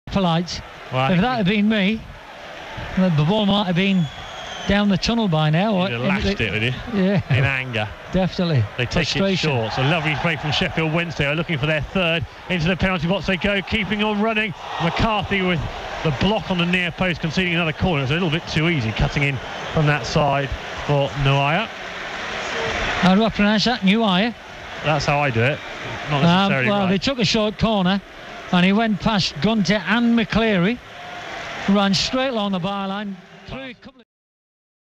Dazzling skills! | Atdhe Nuhiu takes on Reading!
Atdhe Nuhiu demonstrated some tricky footwork against Reading on Saturday as Sheffield Wednesday defeated Reading 2-0 at The Madejski.